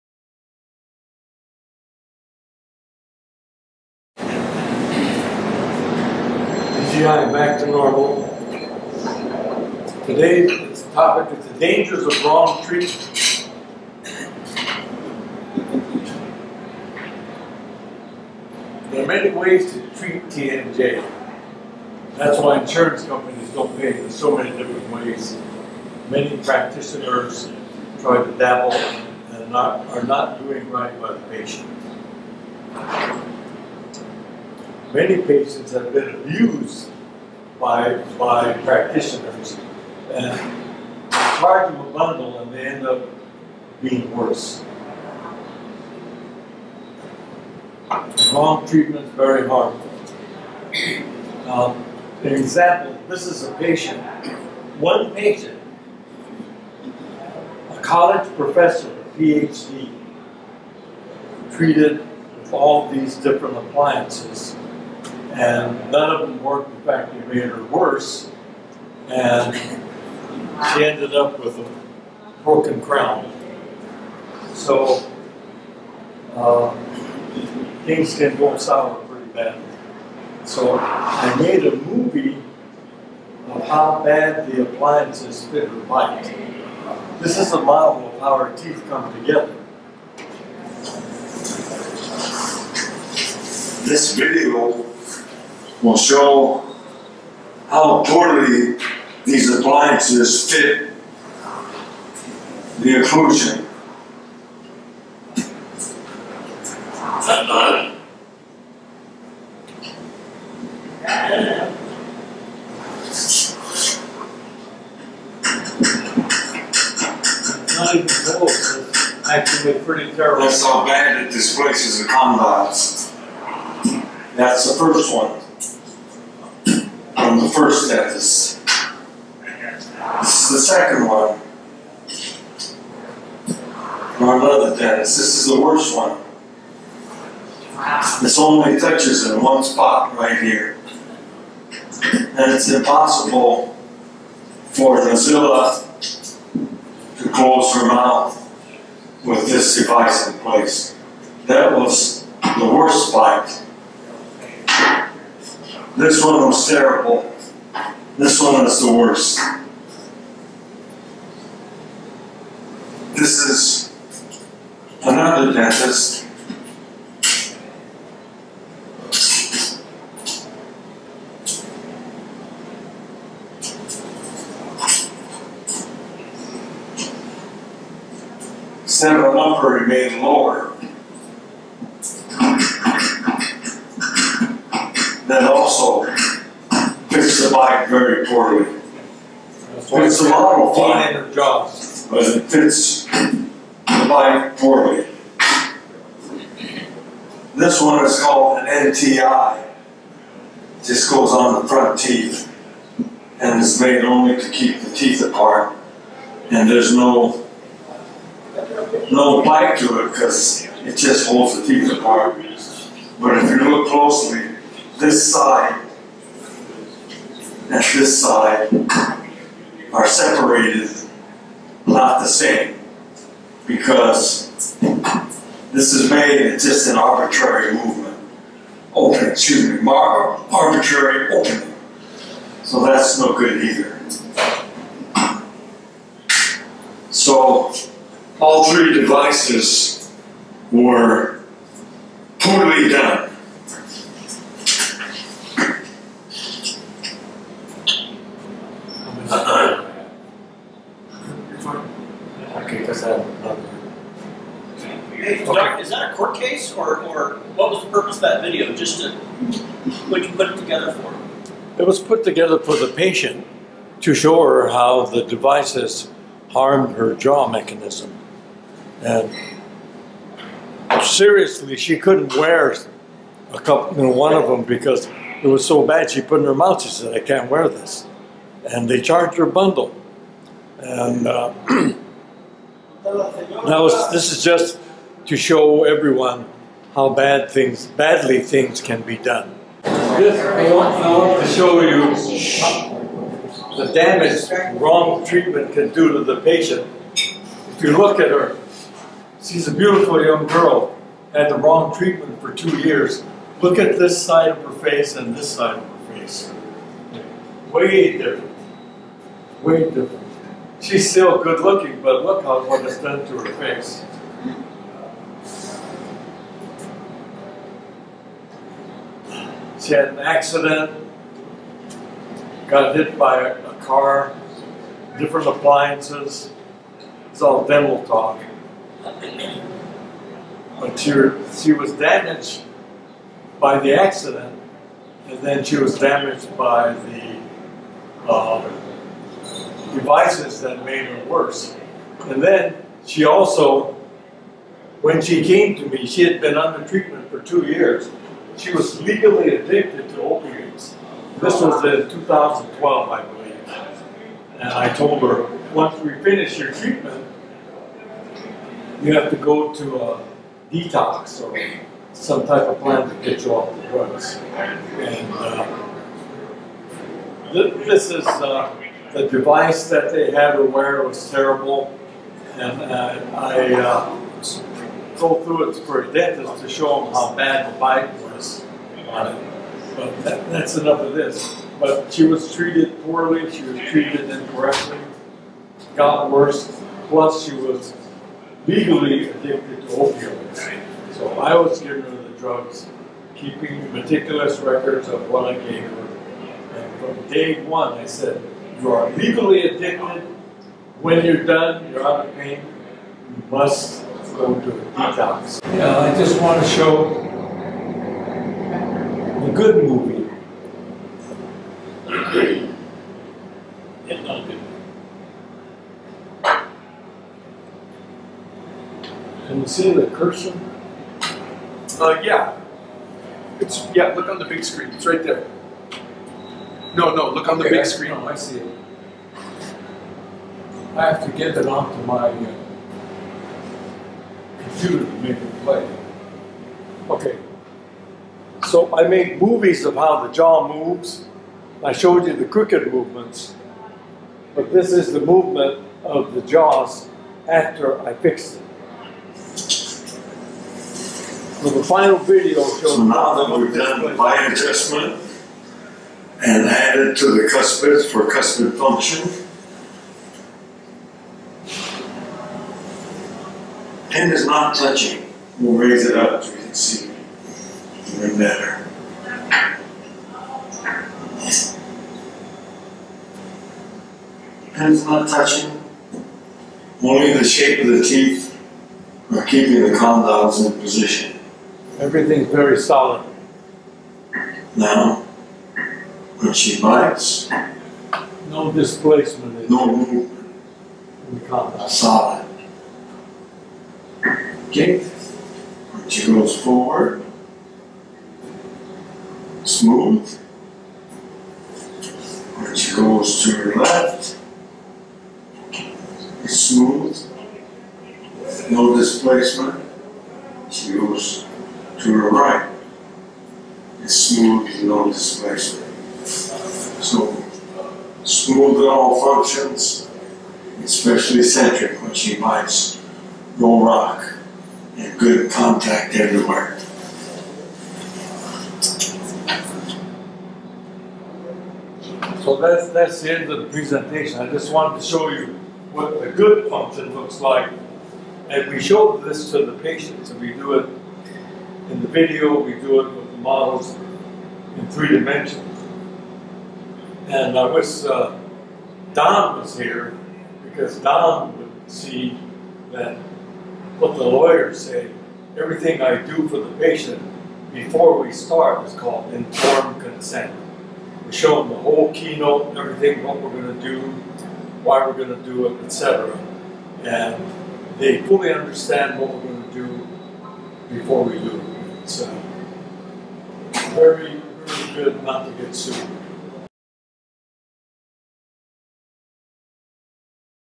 How improper TMJ/TMD treatments can actually make a patient WORSE and how to help them
Dr. Mark Z. Yamamoto talks about how improper TMJ/TMD treatments can actually make a patient WORSE and how he can help them by applying the proper techniques of bite adjustment, also known as occlusion treatment.